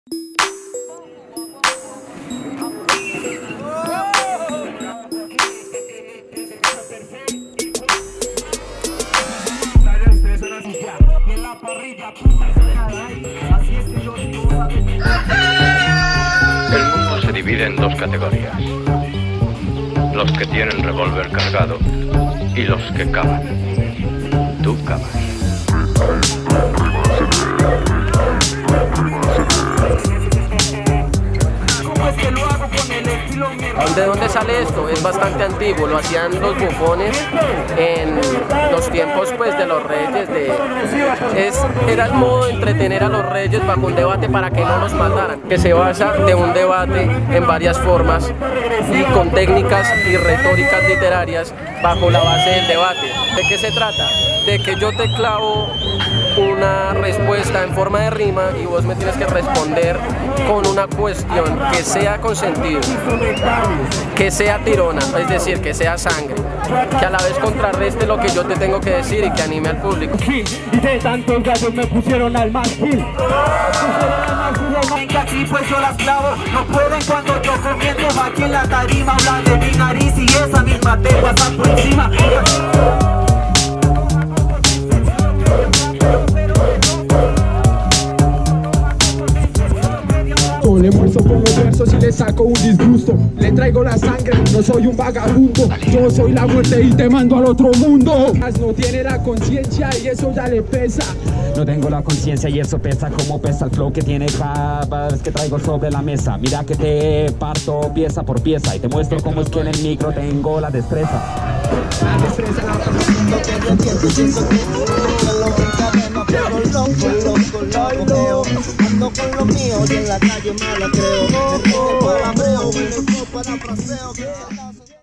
‘Pelea de gallos’ en Bogotá | EL TIEMPO
Jóvenes se reúnen todos los jueves entorno a una `pelea de gallos´, donde con rimas y pullas quieren demostrar quién es el mejor de la ciudad.